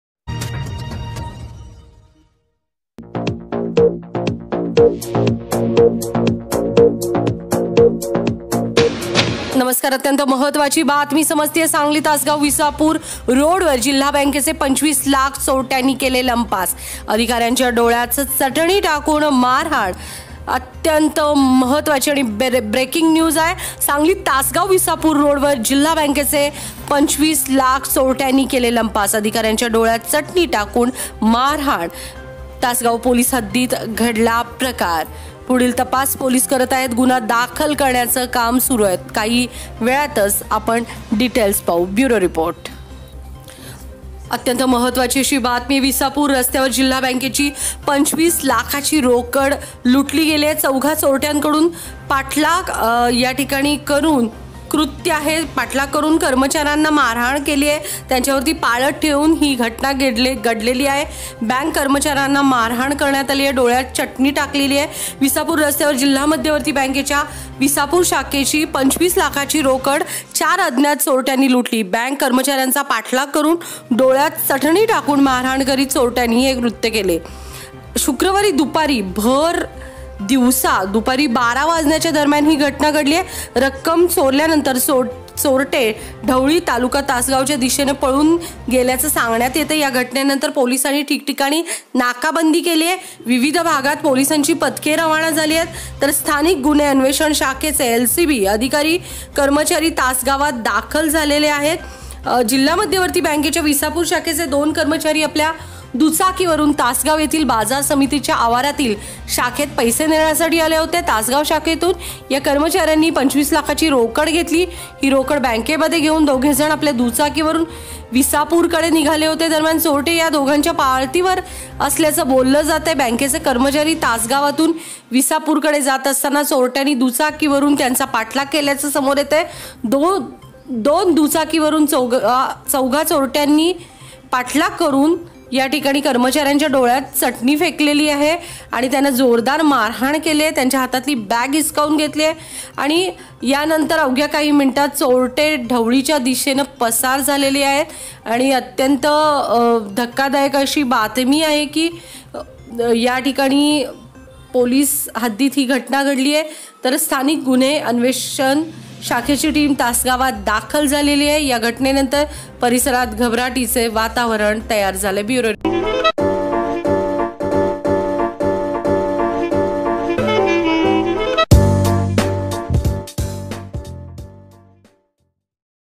तासगाव विसापूर रस्त्यावर जिल्हा मध्यवर्ती बँकेचे 25 लाखांची रोकड अज्ञातांनी लुटली. sharvari pawar
ती चा जागर director